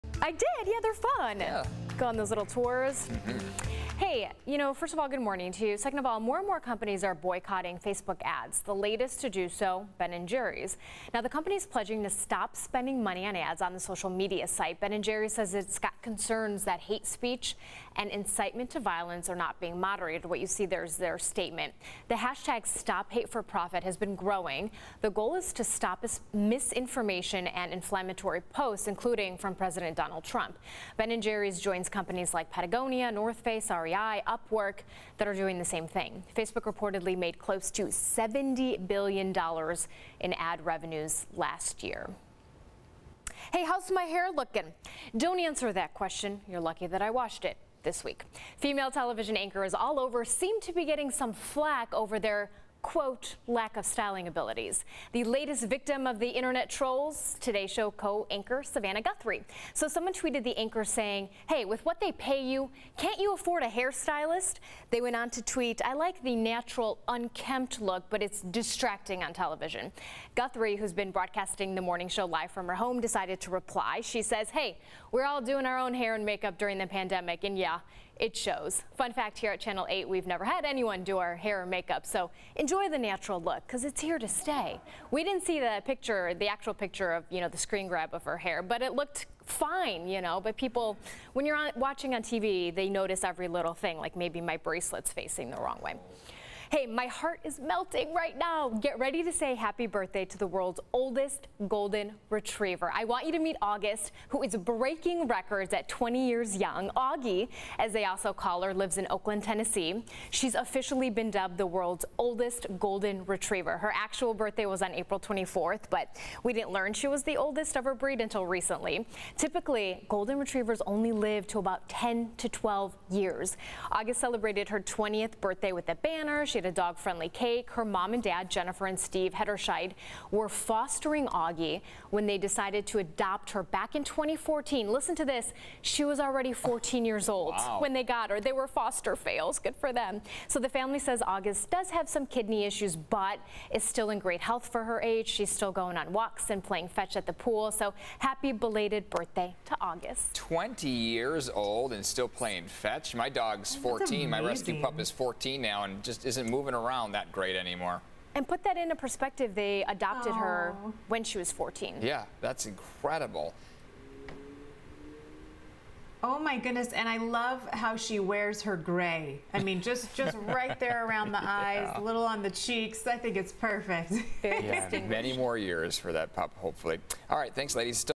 Top trending stories for June 25
This update includes another political stance from an ice cream maker, trolls come after Savannah Guthrie & the world's oldest golden retriever is celebrating a bday